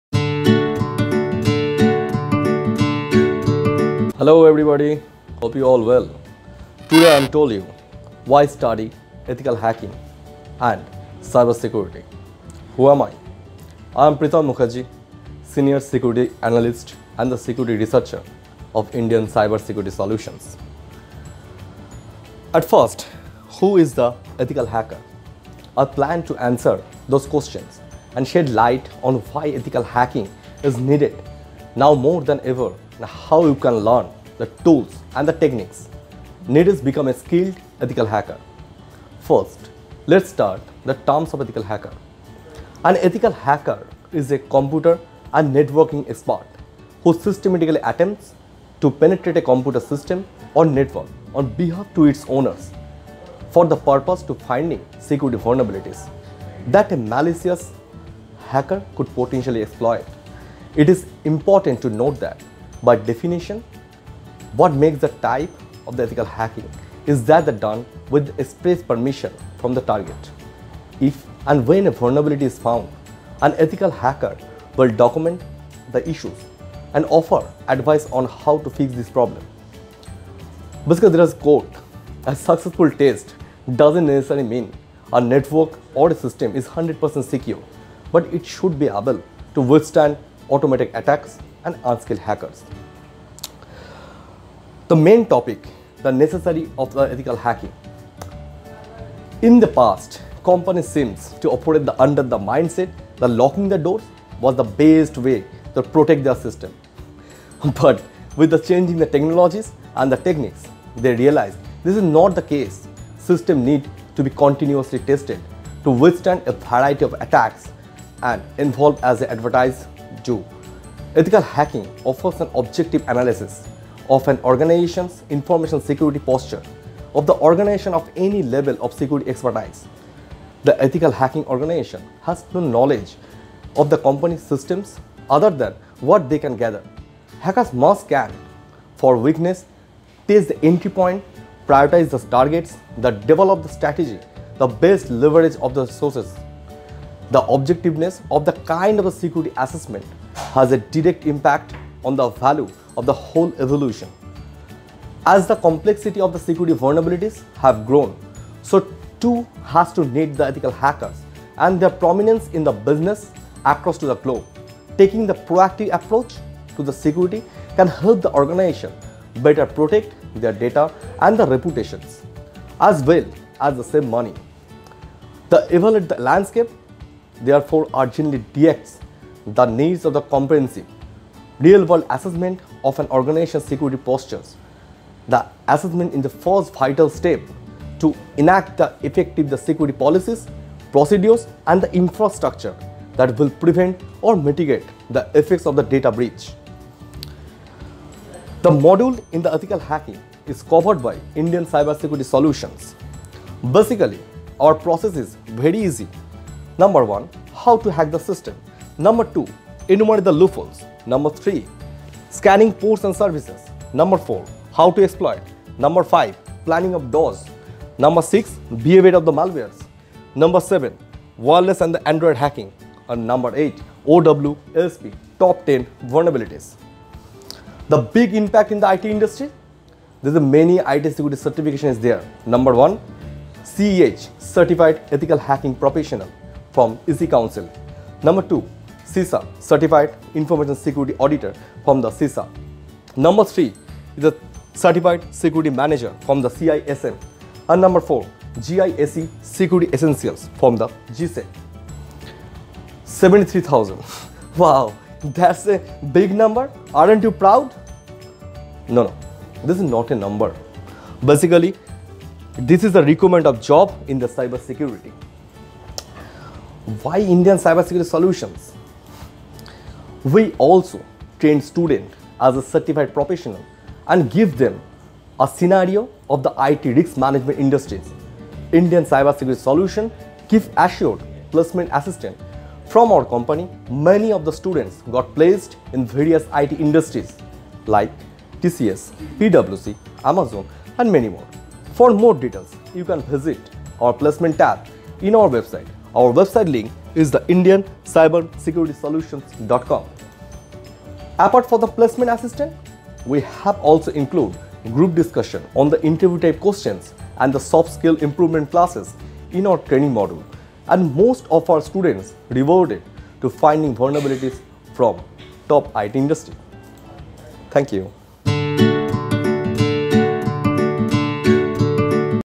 Why study ethical hacking and cyber security? - Indian Cyber Security Solutions
why should we study ethical hacking and cyber security ?

lets meet Senior IT Security Analyst of Indian Cyber Security Solutions, Pritam Mukherjee what he has to speak about this topic...he is going to explain in full details about ethical hacking and cyber security...He also will explain the importance of ethical hacking training...he is going to say in details all the topics that are covered in Indian cyber security solutions.